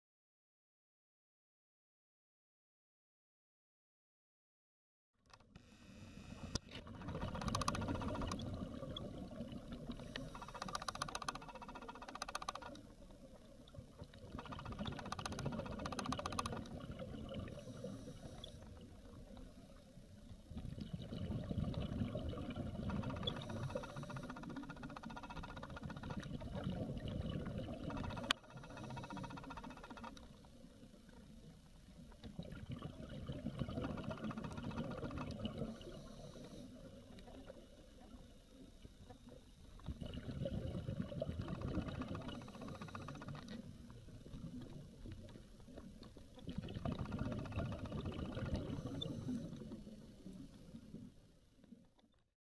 マツカワ 2014/11/09 函館・臼尻
幻の魚と言われているマツカワ（マツカワガレイ）、高級魚で食べると美味しい（らしい）。
捕れる数が少ないため人工種苗した稚魚の背びれにタグを付けて放流しているそうですが、このマツカワには
タグが有りません。もしかしたら天然もののマツカワ？　お腹が黄色いのはオスだそうです。